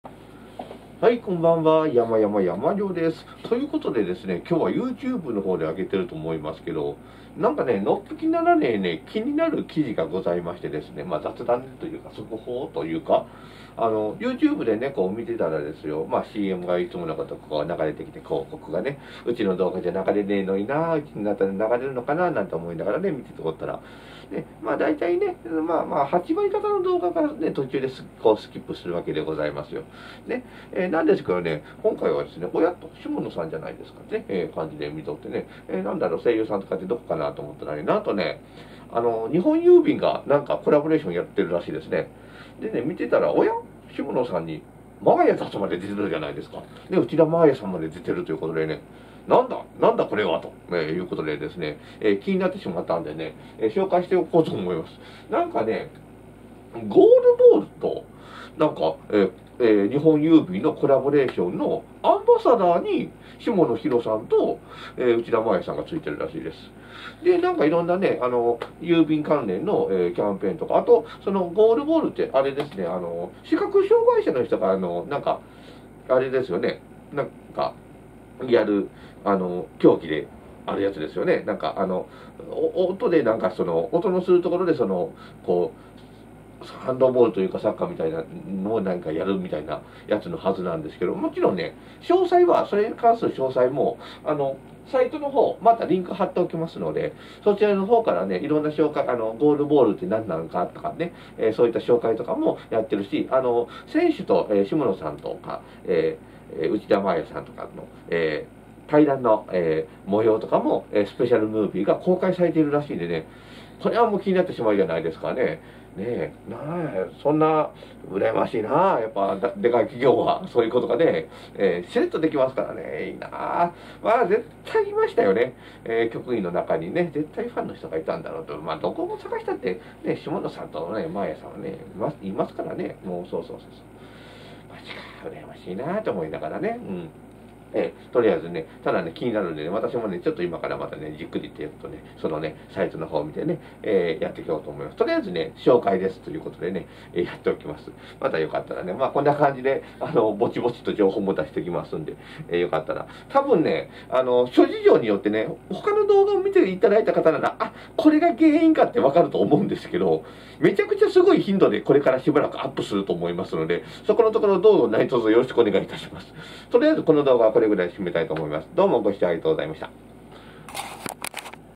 ゆうTube25：日本郵政ゴールボール応援プロジェクトのアンバサダーに下野紘さんと内田真礼さんが就任されたそうです
パラリンピックの競技としても注目度が高いゴールボールを日本郵政が応援しているプロジェクトらしいです。

各種キャンペーンやボイスドラマ・選手と声優さんとのスペシャル対談なども公開されていますよ！